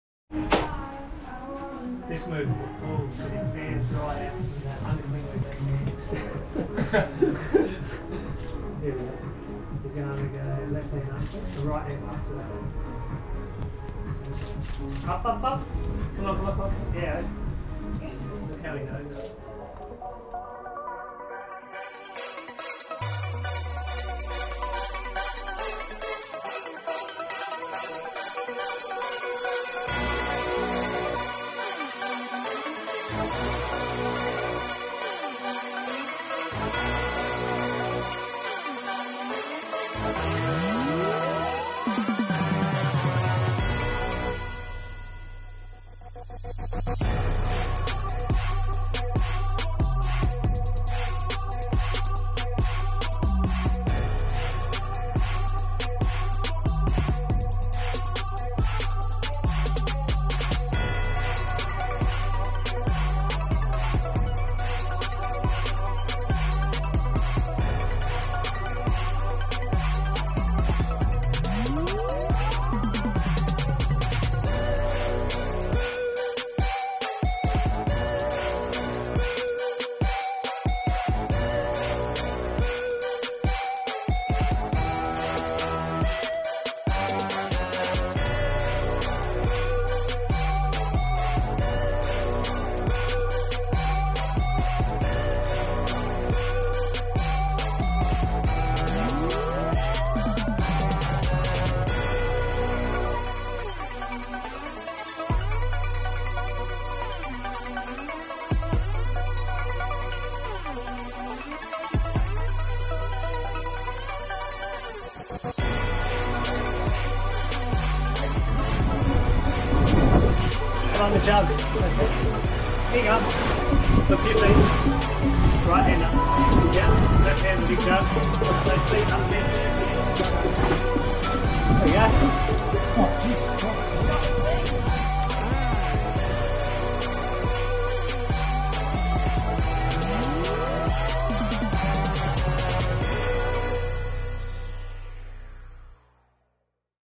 Big O with a Big Send
Friday 5th Jan, owen sending a V9? at Carrickalinga!!!